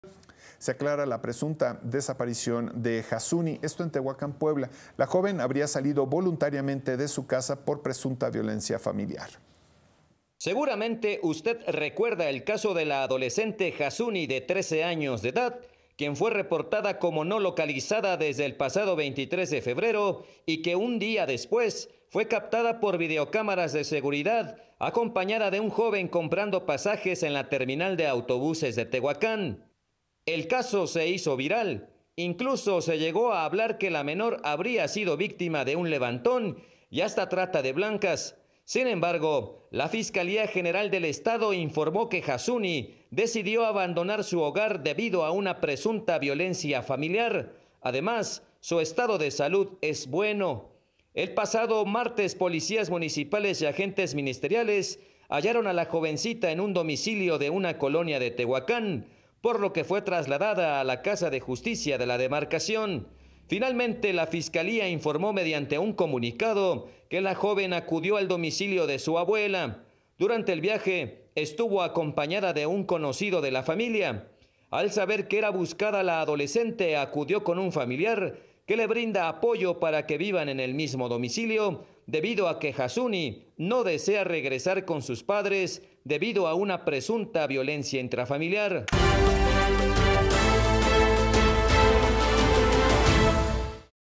Huyó de su casa por presunta violencia familiar | Las Noticias Puebla - Noticias en vivo en Puebla
Se aclara la presunta desaparición de Jazuni, la joven salió voluntariamente de su casa, por presunta violencia familiar.